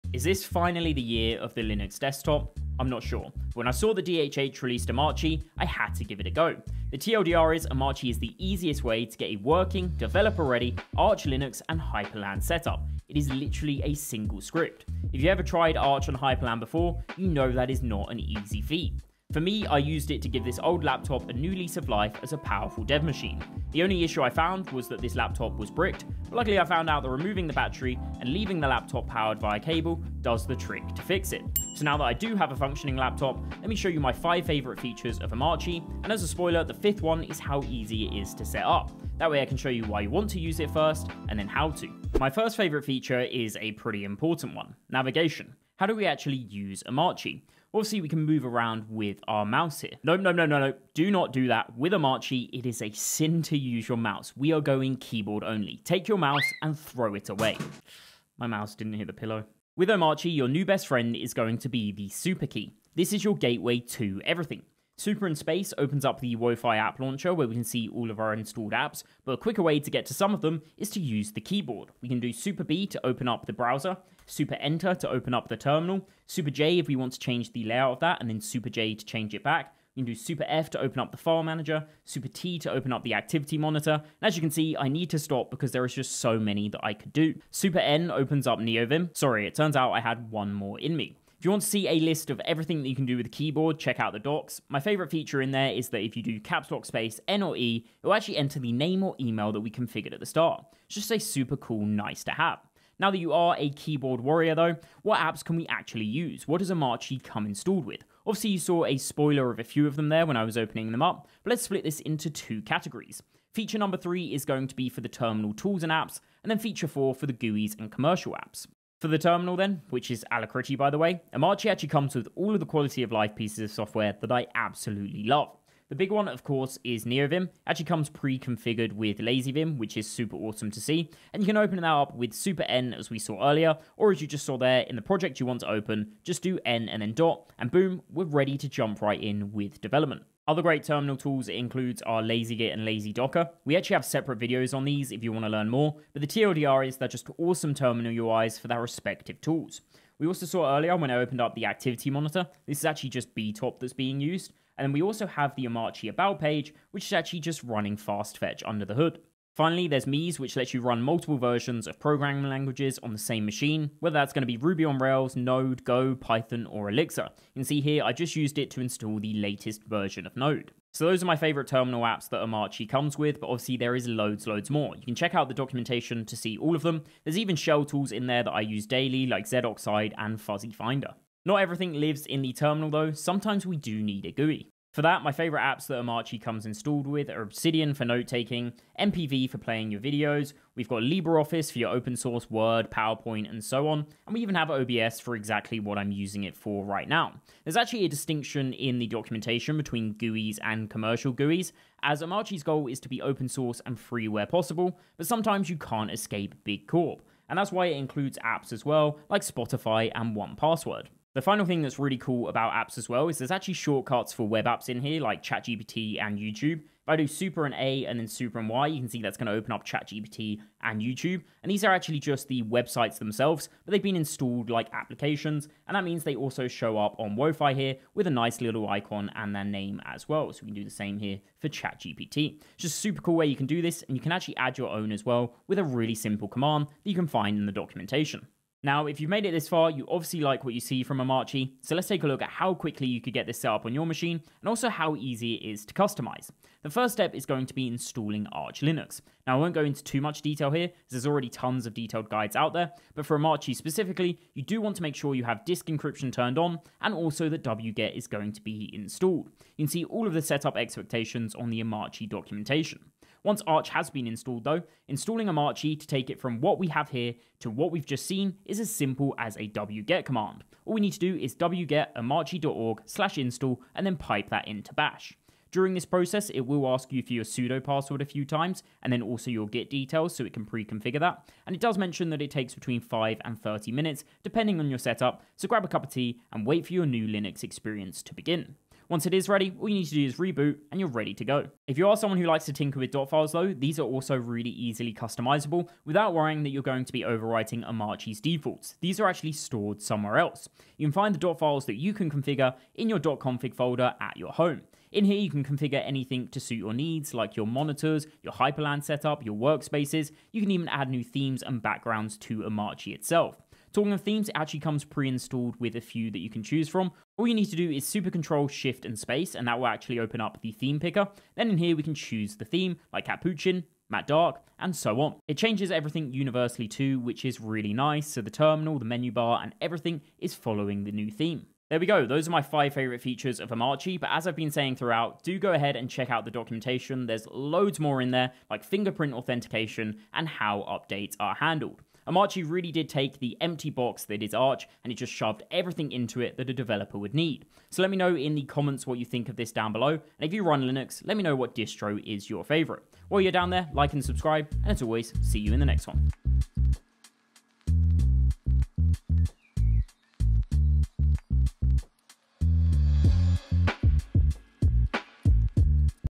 The ULTIMATE Arch Linux + Hyprland Dev Setup – Omarchy
DHH's Omarchy just made Arch Linux + Hyperland setup ridiculously easy with a single script install. I tested it on an old bricked laptop and discovered 5 game-changing features that make this the most developer-friendly Linux experience I've ever used.

📌 Chapters:
0:00 Intro
0:42 Navigation
1:43 Terminal Apps
3:01 GUI Apps
3:59 Easy to Setup
4:52 Customisable
5:32 Whats your Linux Distro?